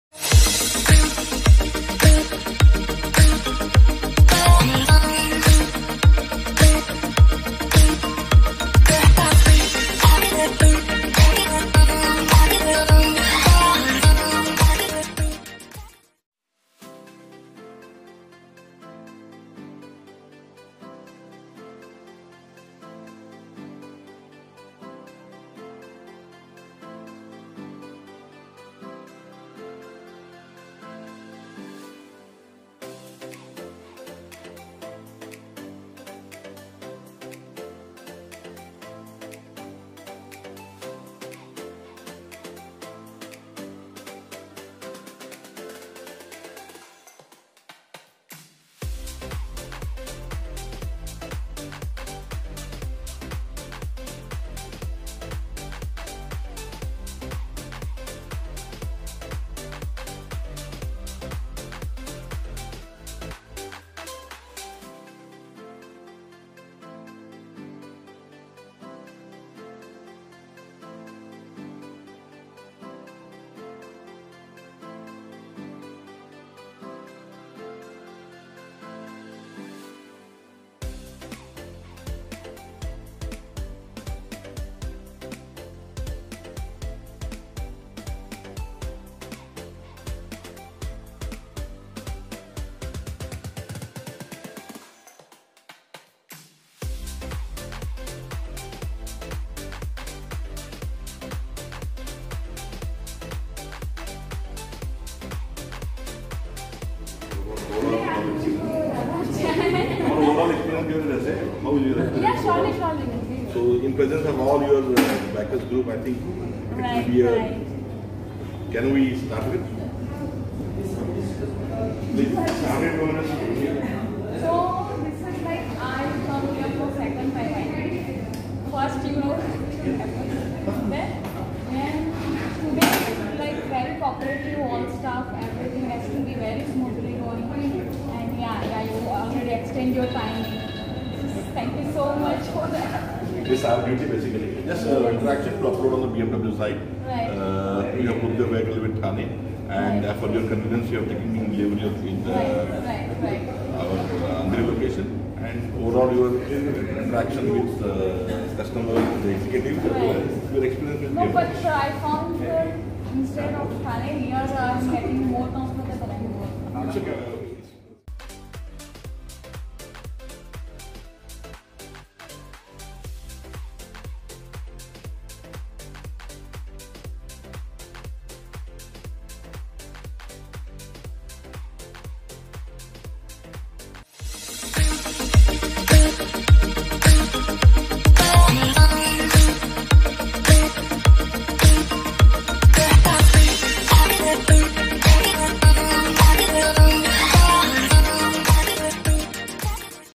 Sonal get her BMW 310R 2160p
So, we, members of the Biker Brotherhood Club join our dear rider Sonal as she gets the delivery of her brand new BMW 310R from NavneetMotors in Andheri.

It is an amazing experience as the staff is just awesome and we didn't feel out of the place for a moment. We were in the 7th heaven as we checked out all models of BMW Motors up and close.